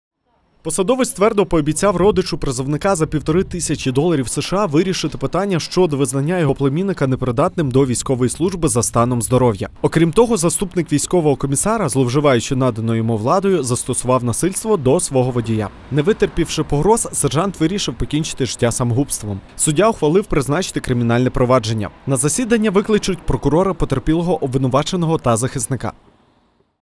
У Хмельницькому судять колишнього заступника військкома. Перший Подільський 21.04.2021
Хмельницький  міськрайонний суд розглянув обвинувальний акт стосовно заступника військового комісара Хмельницького об’єднаного міського військкомісаріату. Його обвинувачують у зловживанні впливом та доведенні до самогубства підлеглого.